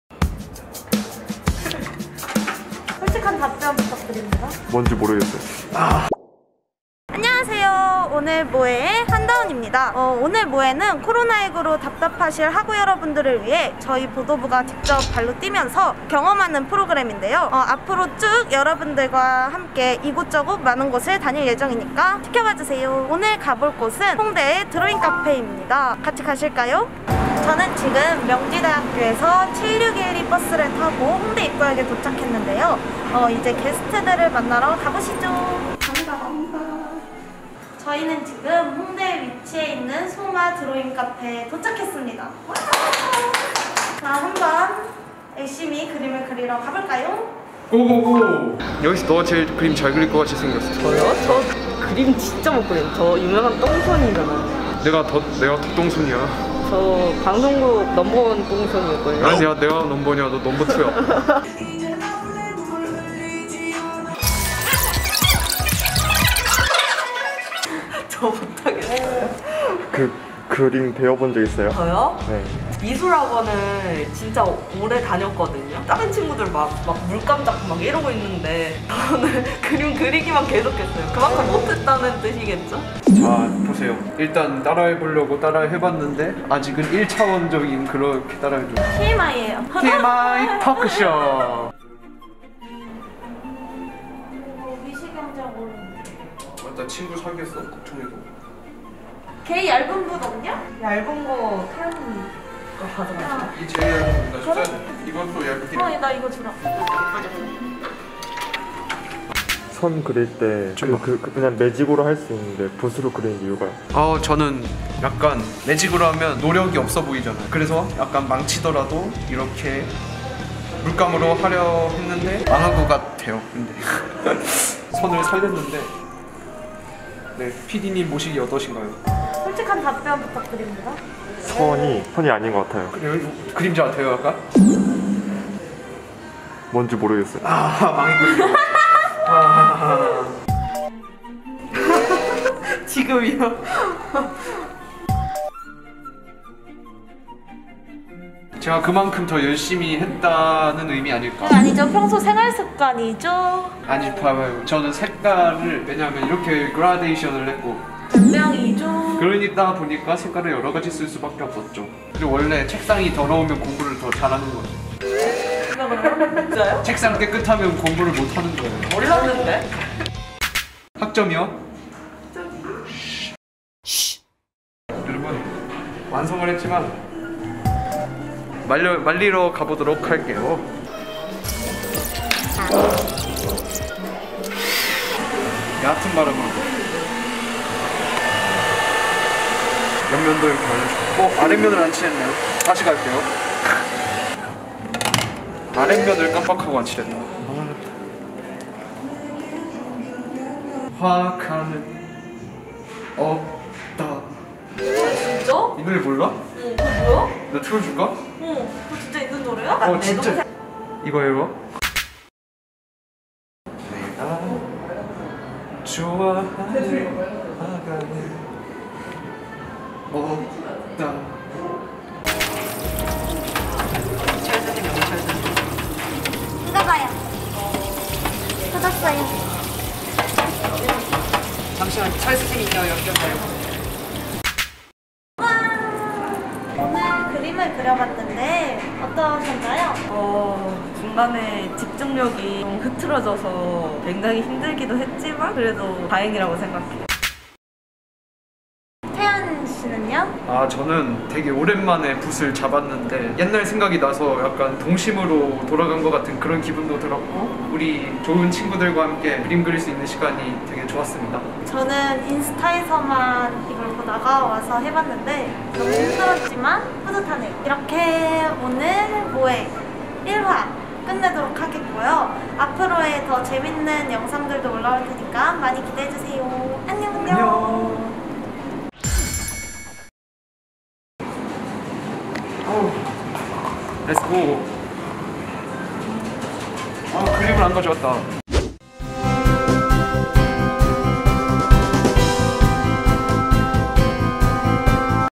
드로잉카페 갔다가 작품 놓고 온 사람이 있겠어??ㅋㅋ 그거 나임.. [오늘 모해!? _ 첫번째 나들이]
명지대학교 보도부와 함께하는 재밌는 나들이!
코로나 19로 답답해하실 여러분을 위해서 저희가 나섰습니다!
오늘은 함께 '드로잉카페'를 가보았는데요!
보도부 다원이의 첫 우당탕탕 드로잉카페 경험기를 함께 보실까요!?

제작,연출 : MBS 보도부 한다원
기술 : 이성빈, 김예진
출연 : 한다원, 신유영, 이태연